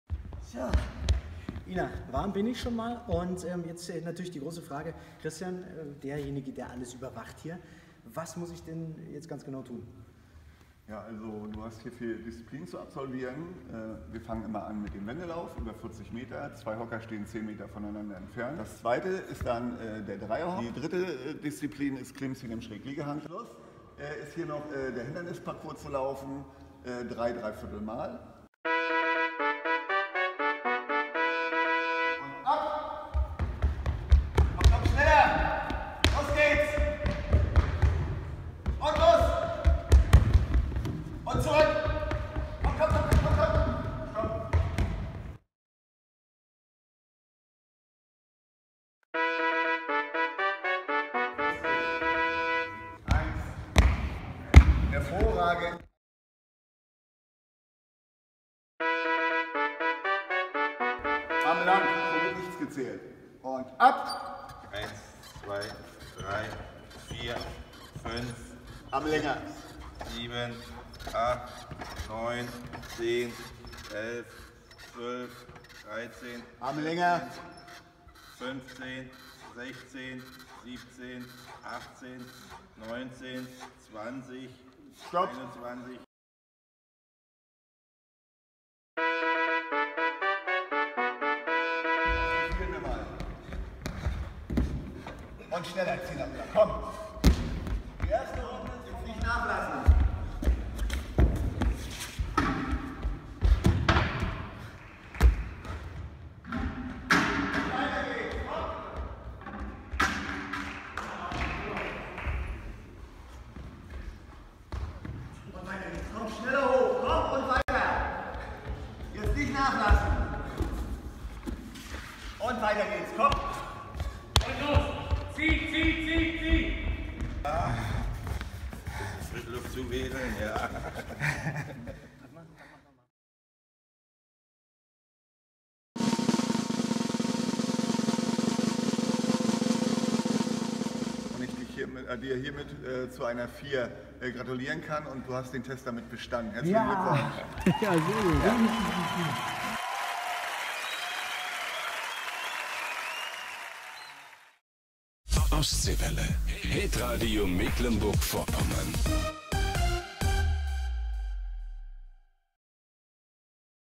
Uwe macht den Polizei-Sporttest
Die Polizei in Mecklenburg-Vorpommern sucht händeringend nach Verstärkung, doch viele Bewerber scheitern am Sporttest. Uwe 1,69einhalb Worlitzer vom "Guten Morgen" ist der Meinung, dass dieser Test doch mit Leichtigkeit zu schaffen sei. Innenminister Lorenz Caffier hat ihn herausgefordert. Doch schau selbst im Video.